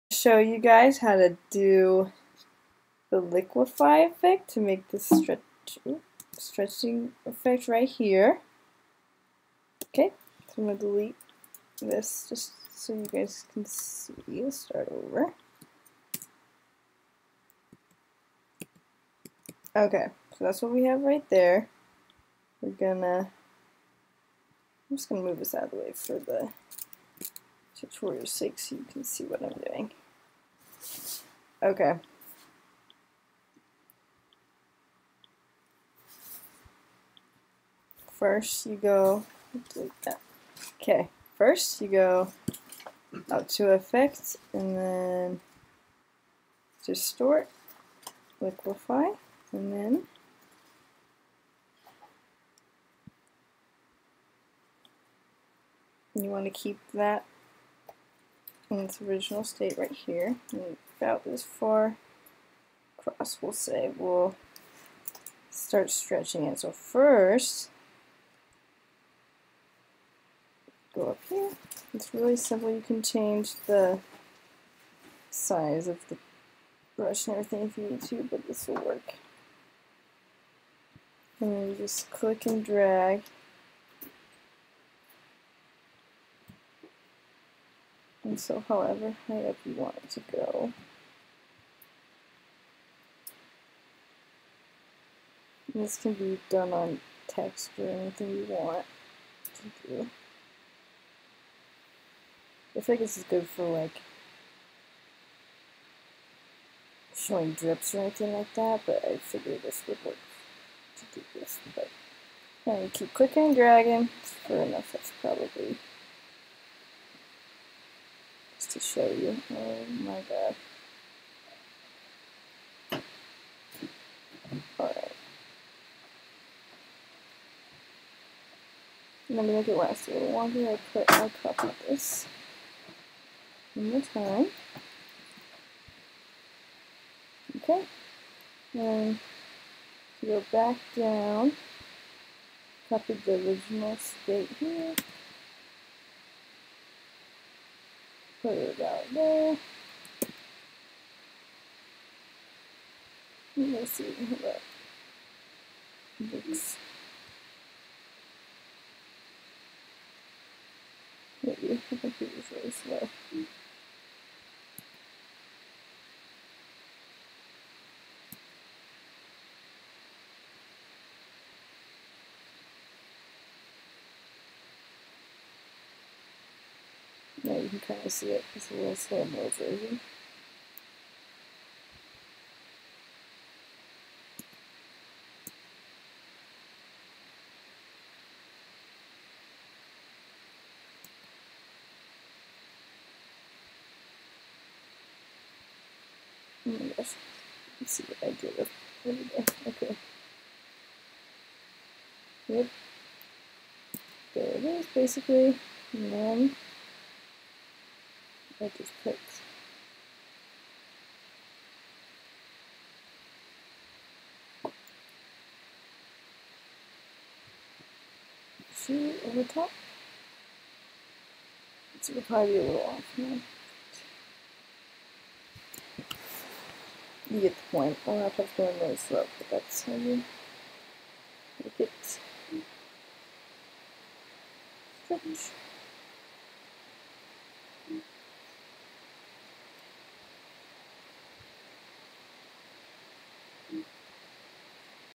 After Effects Tutorial: Liquify Effect
In this video, Kara Sloane covers how to use the liquify tool to stretch an object.